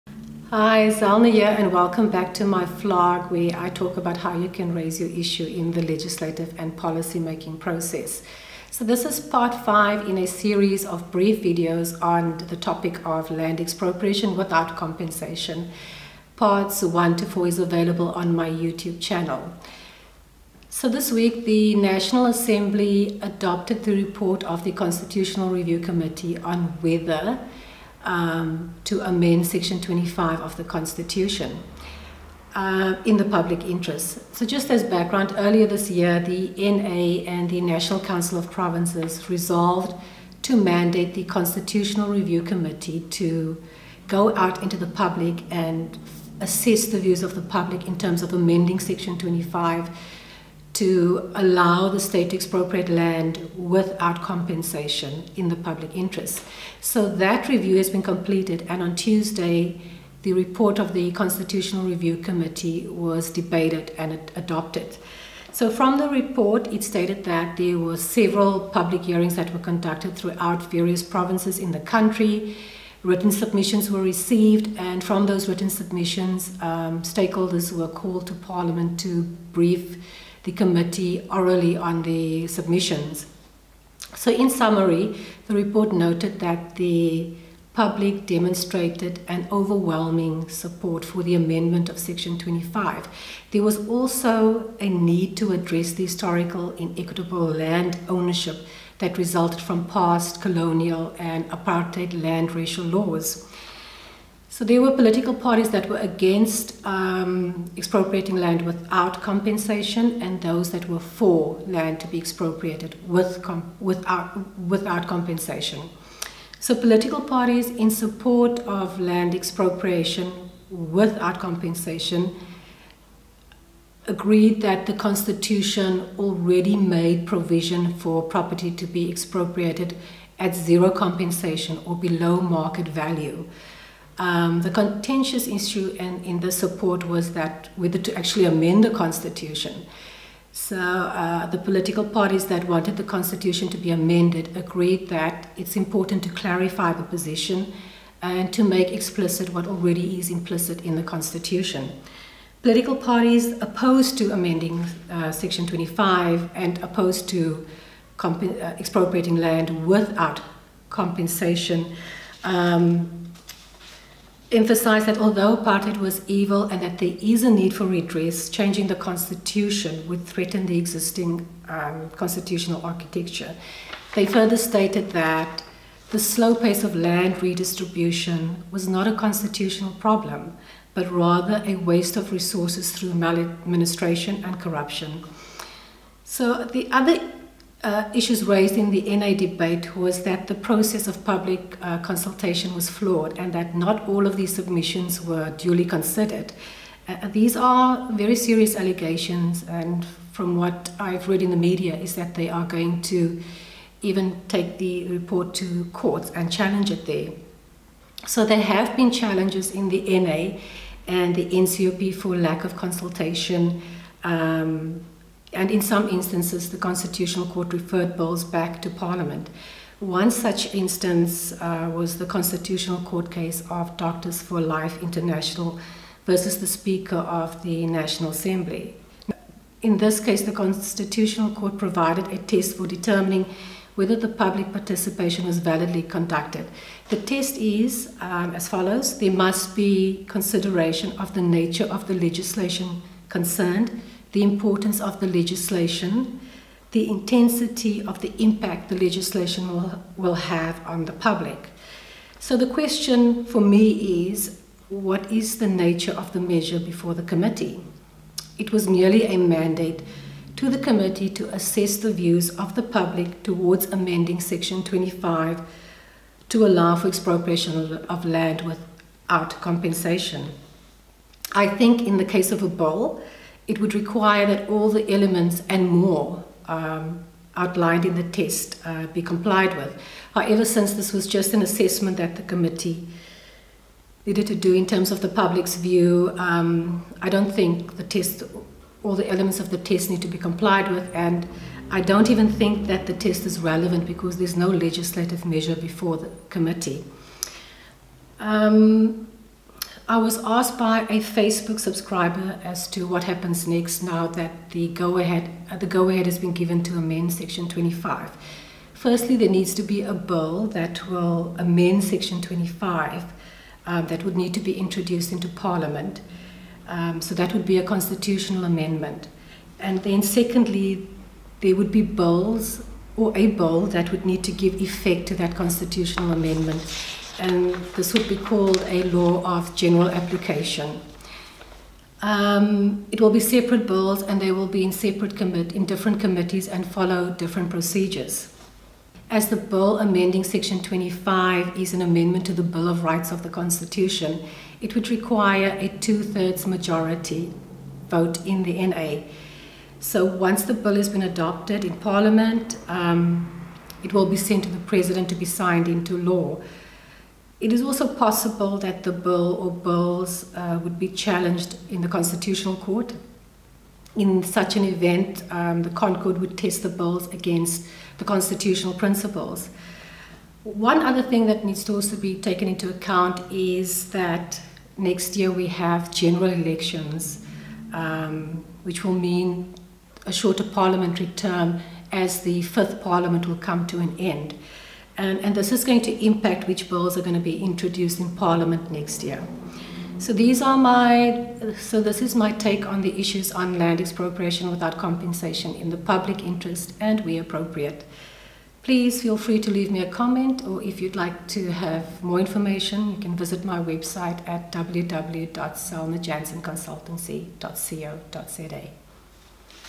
LAND EXPROPRIATION WITHOUT COMPENSATION PART FIVE
In this video, I give a summary and analysis of the issues presently being discussed on the topic of expropriation of land without compensation. I also indicate what the next steps will be in amending Section 25 of the Constitution.